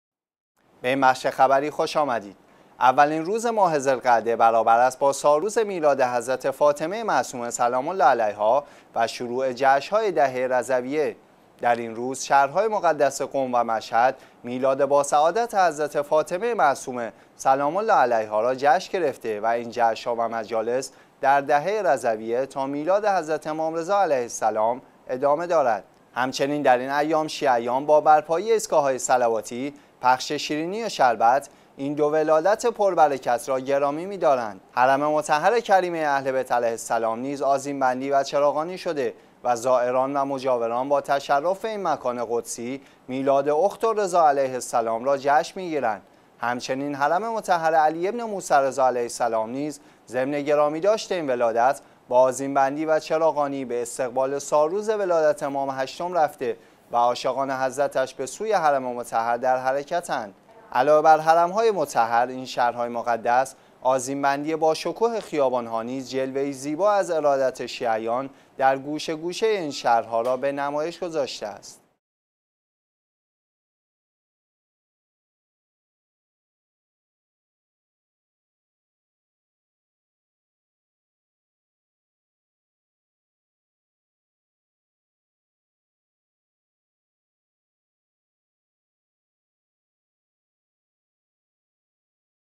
یکم ذی القعده برابر با میلاد حضرت فاطمه معصومه سلام الله علیها
اولین روز ماه ذی القعده برابر است با سالروز میلاد حضرت فاطمه معصومه سلام الله علیها و شروع جشن های دهه رضویه.
در این روز شهرهاى مقدس قم و مشهد میلاد با سعادت حضرت فاطمه معصومه سلام الله علیها را جشن گرفته و این جشنها و مجالس در دهه رضویه تا میلاد حضرت امام رضا علیه السلام ادامه دارد.
همچنین در این ایام شیعیان با برپایی ایستگاه‌های صلواتی، پخش شیرینی و شربت این دو ولادت پر برکت را گرامی می دارند.
حرم مطهر کریمه اهل بیت عليها السلام نیز آذین بندی و چراغانی شده و زائران و مجاوران با تشرف به این مکان قدسی، میلاد «اخت الرضا عليهما السلام» را جشن مى گيرند.
همچنین حرم مطهر «على بن موسى الرضا عليه السلام» نيز ضمن گرامی داشت این ولادت، با آذين بندى و چراغانى به استقبال سالروز ولادت امام هشتم رفته و عاشقان حضرتش به سوی حرم مطهر در حرکتند.
علاوه بر حرم های مطهر اين شهرهاى مقدس، آذین بندی با شکوه خیابان‌ها نیز جلوه‌ اى زيبا از ارادت شيعيان در گوشه گوشه این شهرها را به نمایش گذاشته است.